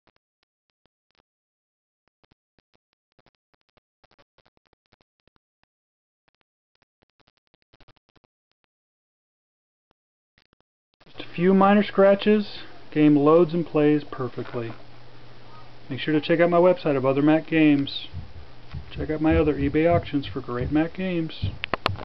Command & Conquer Generals Deluxe Edition - Mac - DVD
Macintosh game "Command & Conquer Generals Deluxe Edition" for sale on eBay. DVD/Manual.